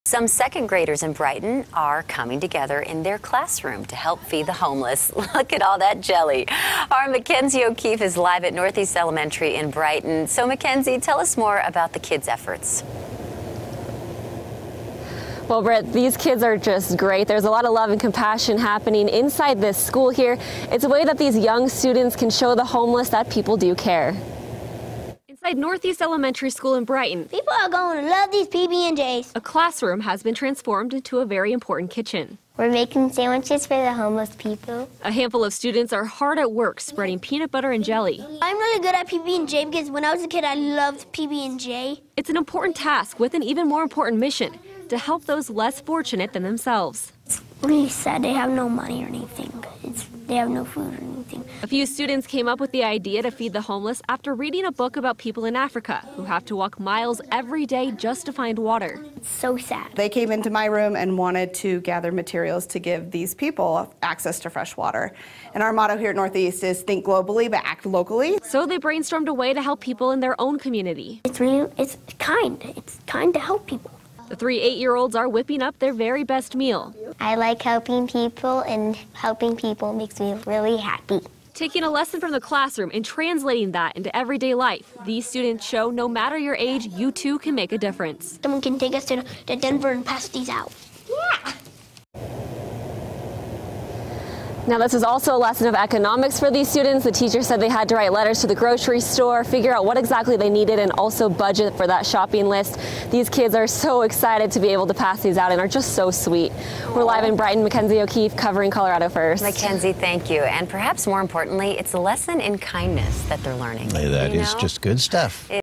2nd Graders In Brighton Helping Feed The Homeless
The students at Northeast Elementary School wanted to help others in need.